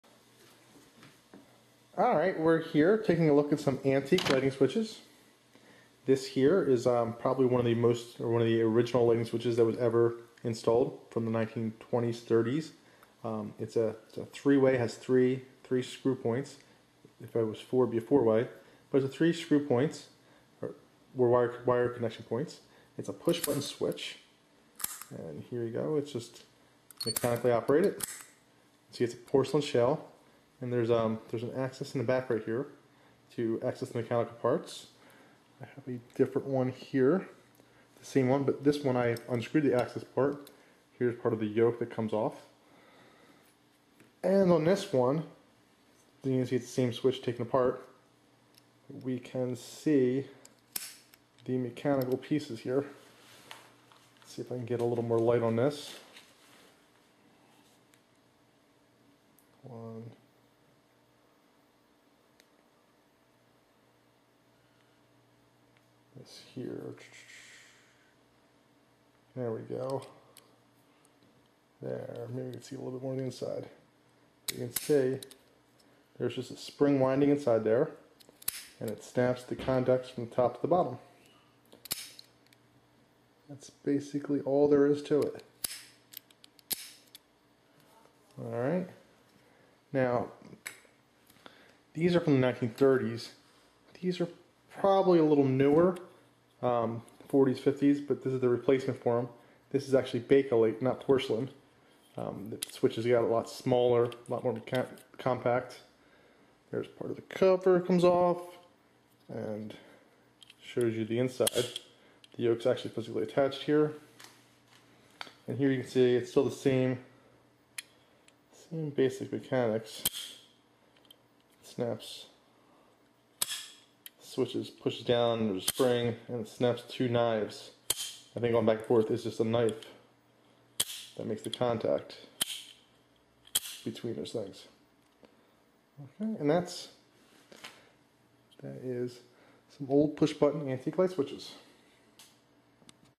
Push button 3way old light switches
A look at the mechanics of old light switches. There are antique porcelain push button light switches